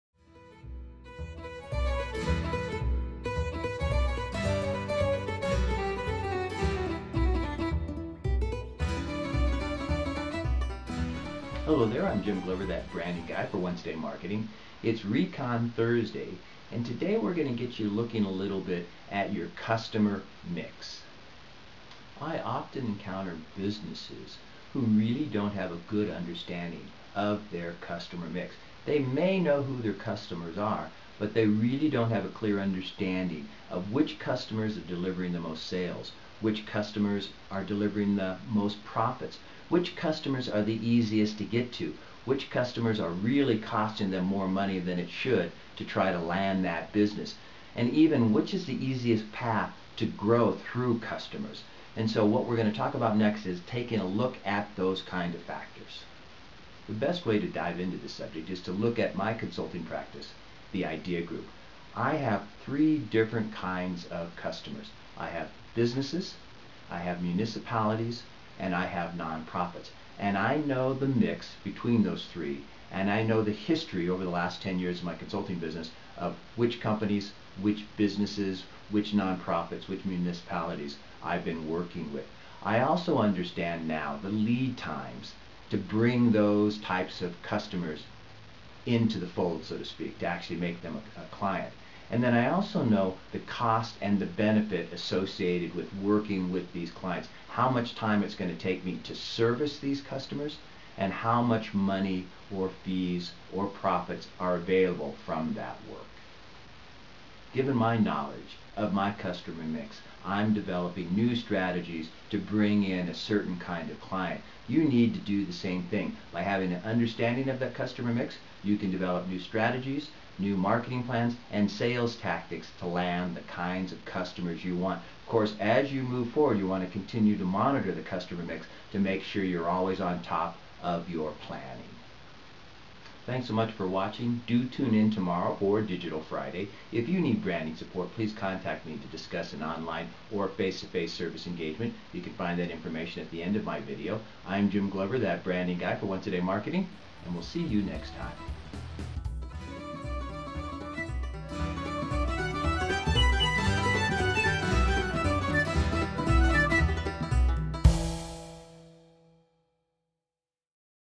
Understanding Your Customer Mix (Recon Thursday/Process)
How well do you know your customers? James Glover, That Branding Guy, discusses how important it is for successful planning to really understand all about your customers in his Once a Day Marketing video advice. Quick Glimpse: Which customers are the quickest path to profitability?